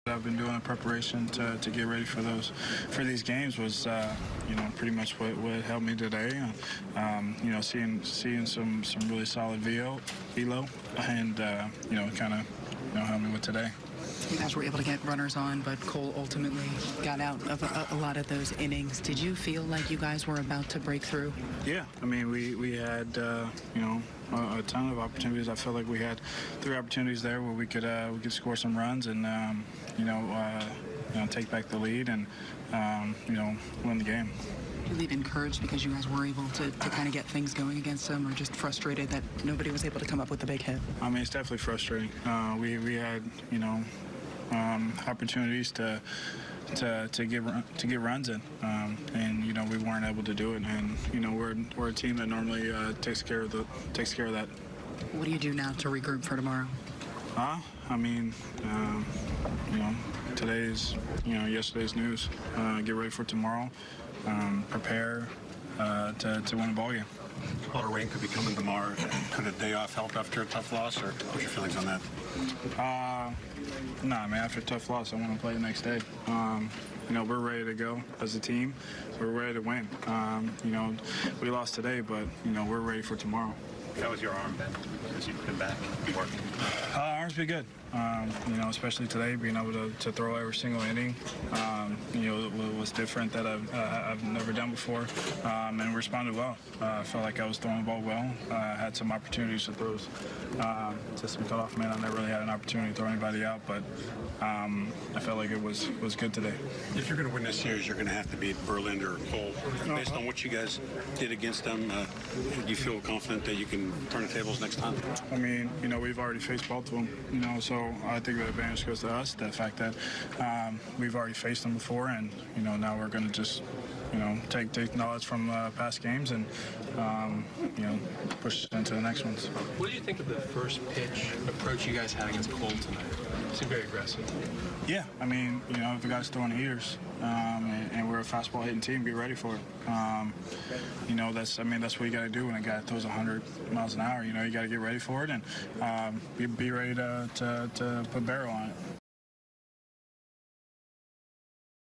Aaron Hicks recaps Yankees' 4-1 loss vs. Astros
Aaron Hicks was back in the starting lineup for the first time since early August and drew a pair of walks as the Yankees came up just short at home against Houston.